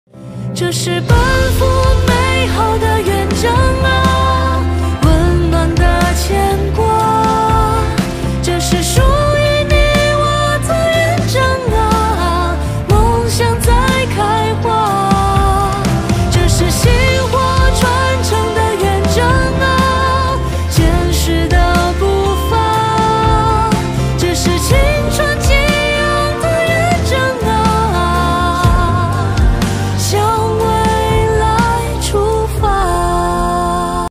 Fairground Outdoors Flying UFO Disk'o Coaster Ride
Capacity:24p
Power:55kw
Height:7.6m
Cover area : 10*30m